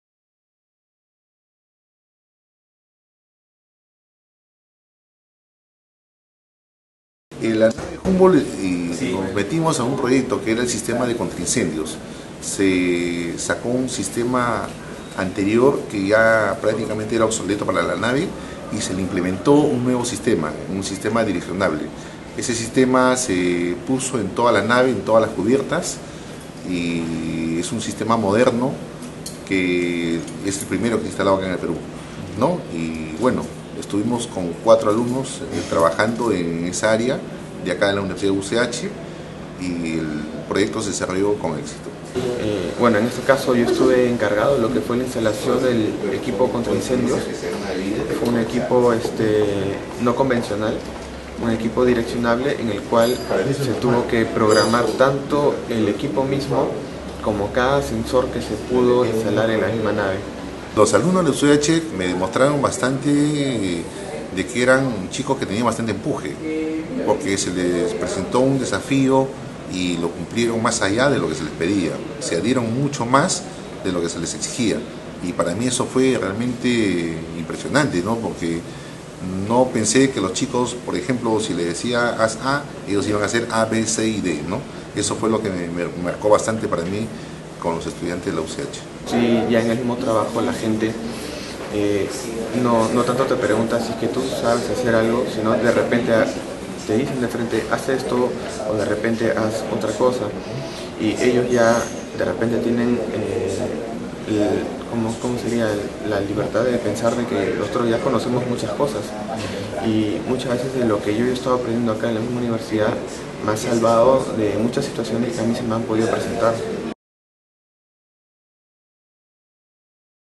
Alumnos de la UCH participaron en implementación del BIC Humboldt
Antes de su partida el buque  fue modernizado  en su totalidad. La implementación del sistema contra incendio le  fue encargada a la empresa Naval Industria, cuyo gerente Manuel Chávez  contó que en ese proyecto trabajaron  cuatro estudiantes de la EAP.  de  Ingeniería Electrónica con mención en Telecomunicaciones de la Universidad de Ciencias y Humanidades (UCH).